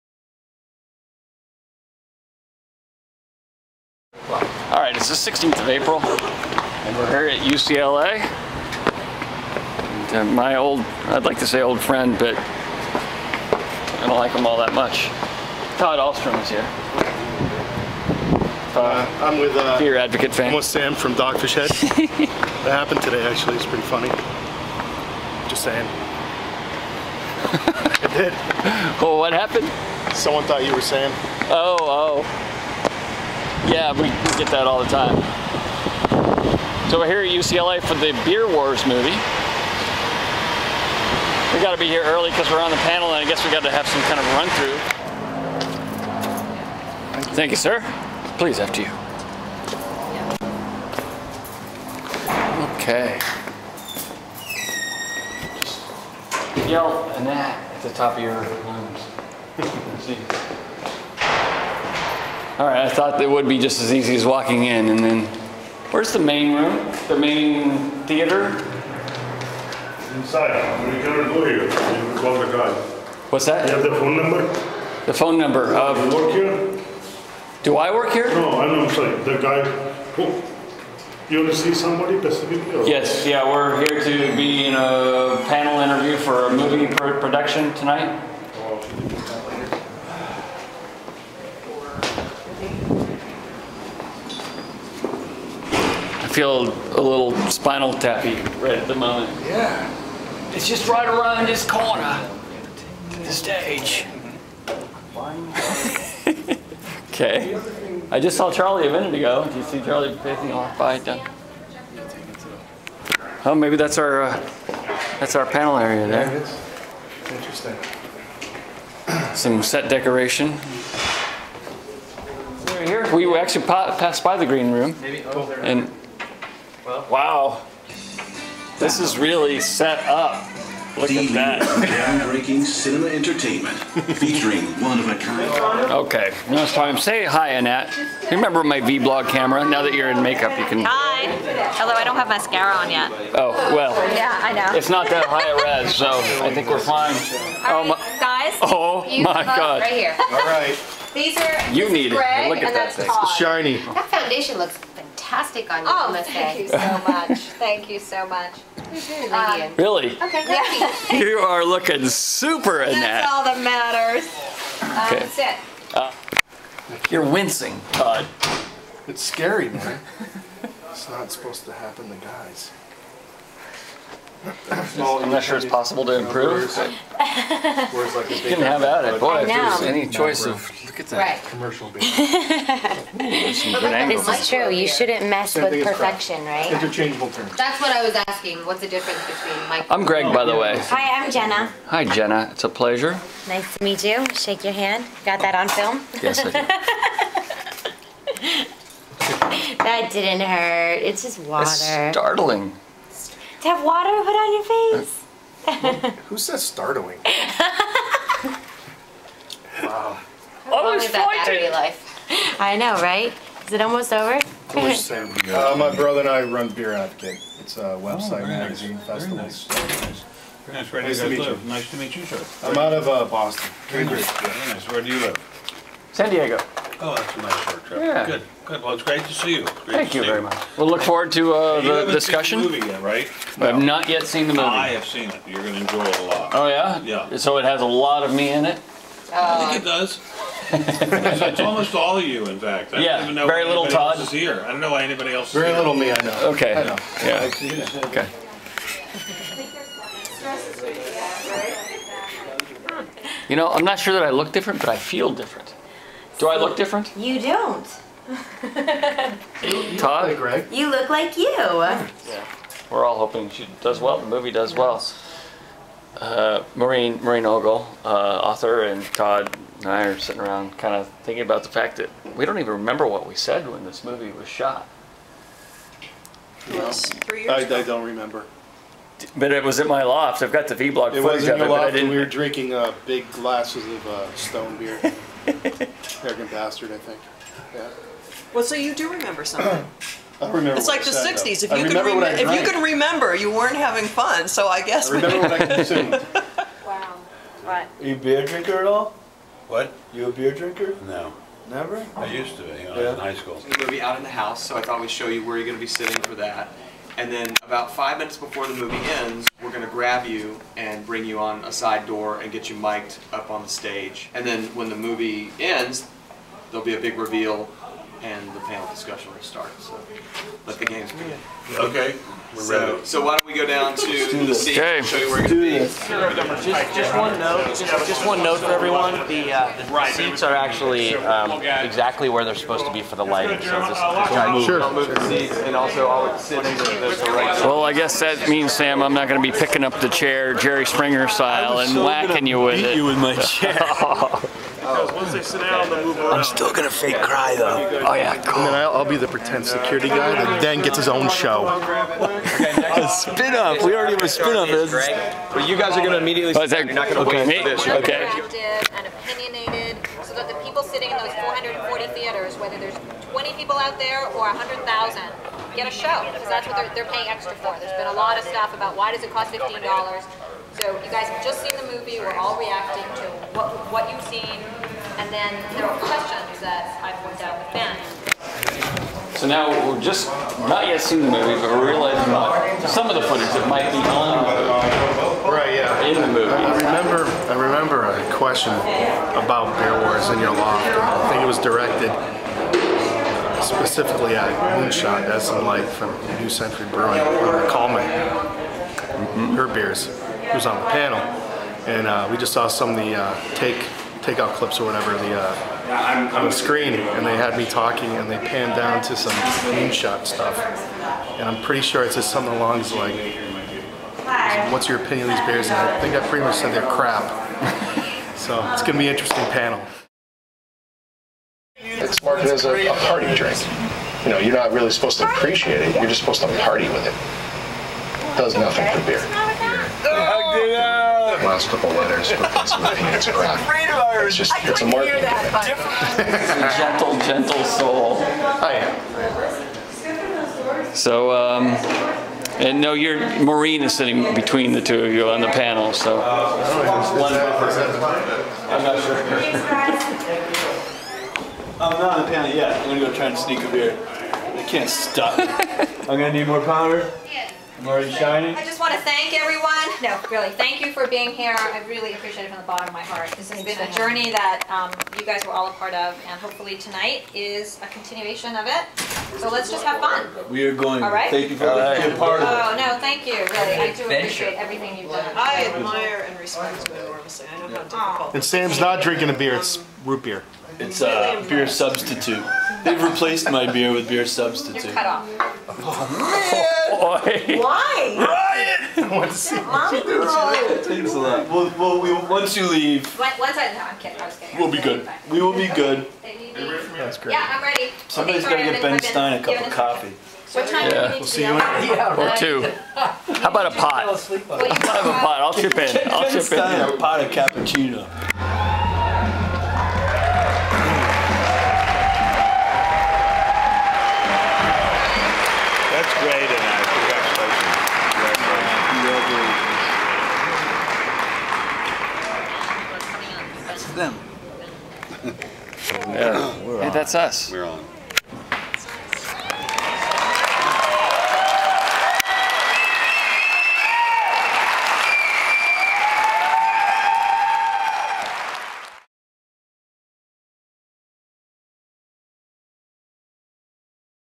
GK vBlog | Backstage at Beer Wars Live
Go backstage with GK and Todd Alström as they prepare for the live discussion panel at the premier of the Beer Wars movie! That is, if they can ever find the stage...